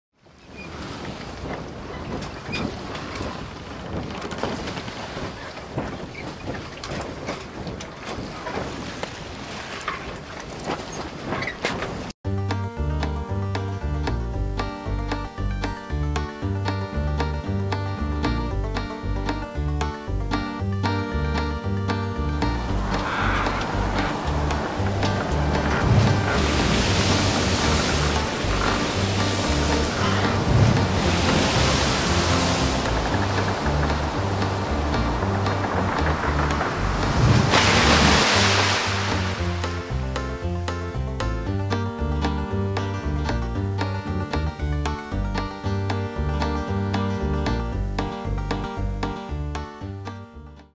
Barack O Pirate Sinks Ship of State in AA+ Hole
Barack Obama hijacks Ship of State, overloads it with gold and debt and sinks it in a blue AA+ hole. His Budget Control Act is declared unconstitutional by Herbert Titus and William Olson.